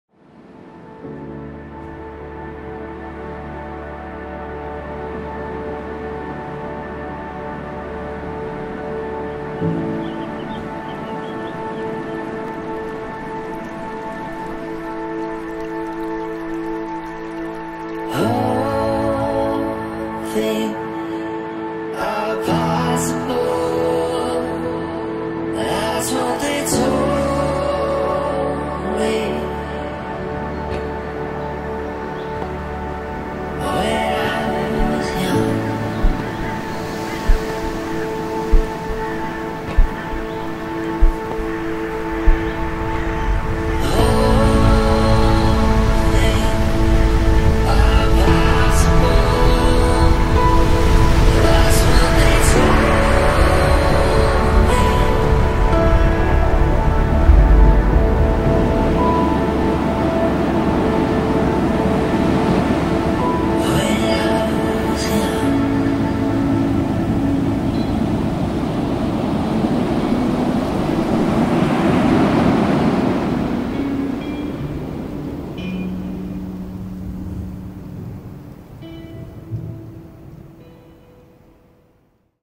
"All Things" - Will Clark
"All Things" is an abstract film based around the idea of the human imagination. As a child, I grew up with a seemingly endless  imagination and would often daydream about the possibilities in life. Although this video was captured from a grounded experience in northern Minnesota, I attempted to add a bit of magic to how I remember the experience. This video was shot in collaboration with Erik Lundstrom, who is an outstanding videographer/photographer. Thank you for the stunning adventure, can't wait for the next one.
                                                     "All things are possible"
Shot by: Will Clark & Erik Lundstrom
Edited by: Will Clark
*I own/have rights to all assets used in this video.